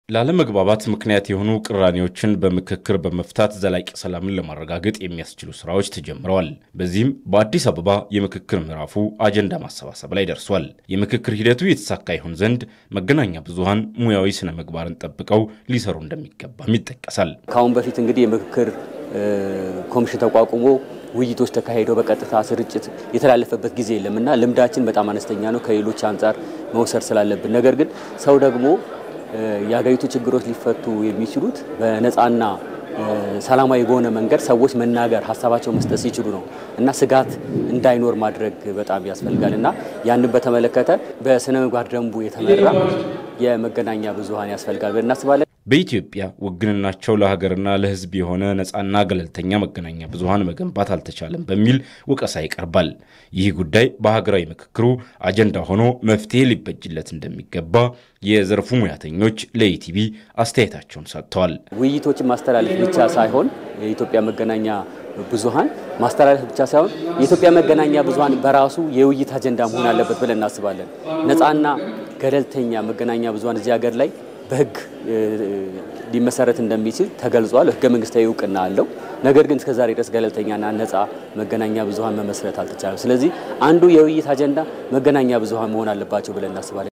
ኢትዮጵያ ነፃና ገለልተኛ መገናኛ ብዙሀን እንዲኖራት በሀገራዊ ምክክሩ አጀንዳ ሆኖ ሊመከርበት ይገባል Etv | Ethiopia | News zena
ኢትዮጵያ ነፃና ገለልተኛ መገናኛ ብዙሀን እንዲኖራት በሀገራዊ ምክክሩ  አጀንዳ ሆኖ ሊመከርበት እንደሚገባ የሚዲያ ባለሙያዎች ተናገሩ።